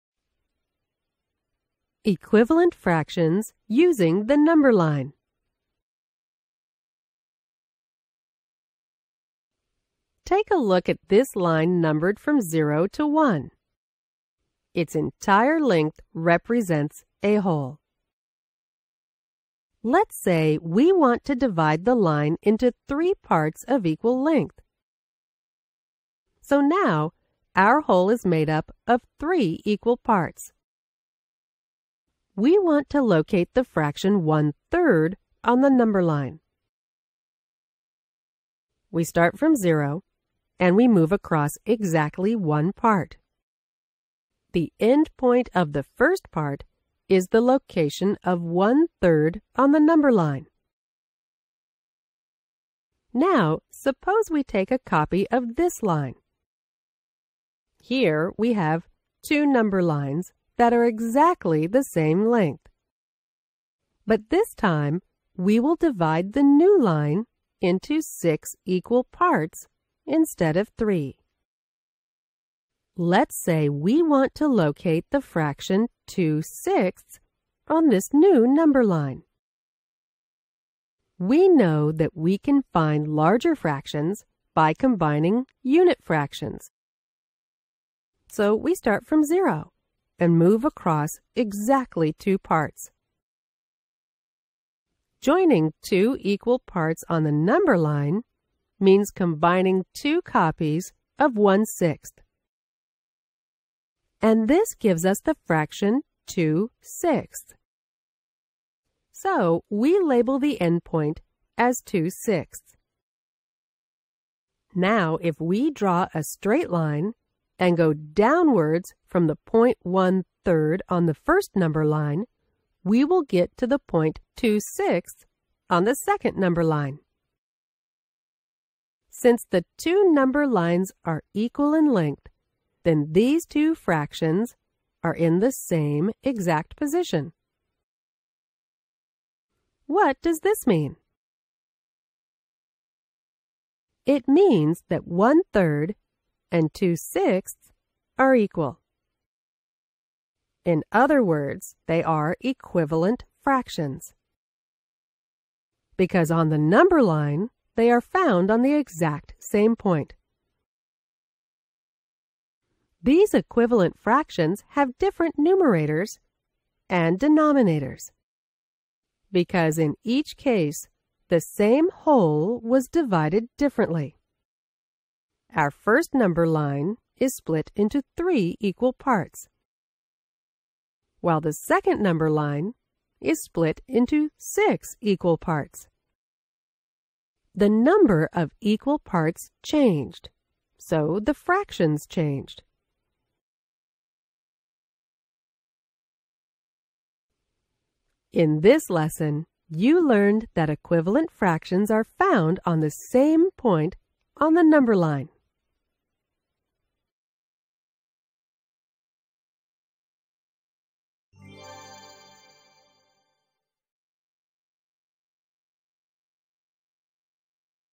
Equivalent Fractions Using the Number Line
Introducing equivalent fractions using the number line.

Now free and open for everyone.